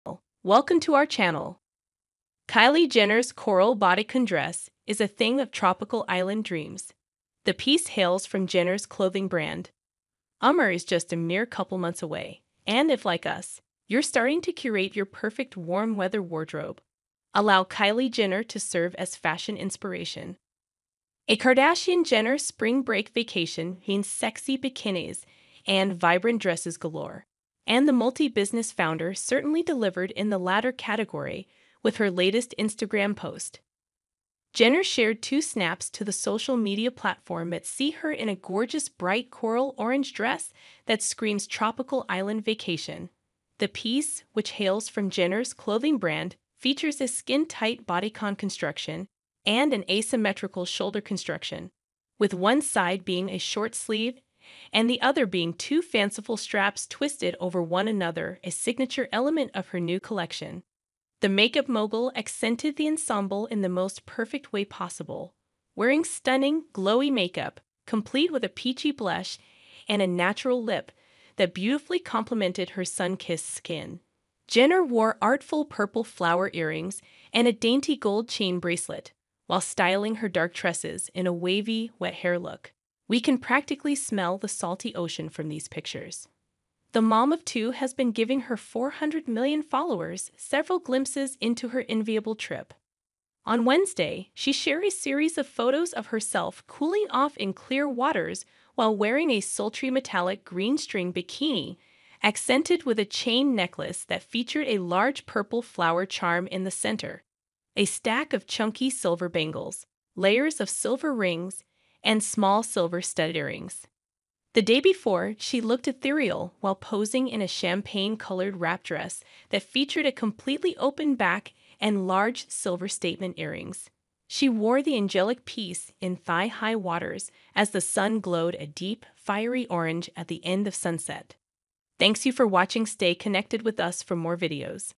Kylie Jenner's coral bodycon dress is reminiscent of tropical island dreams
Kylie Jenner wore a coral bodycon dress from her clothing brand, showcasing her tropical island vacation vibes. The dress features a skin-tight bodycon and asymmetrical shoulder construction, with a short sleeve and fanciful straps. Jenner paired it with glowy makeup, purple flower earrings, and a gold chain bracelet. She also shared photos of her enviable trip.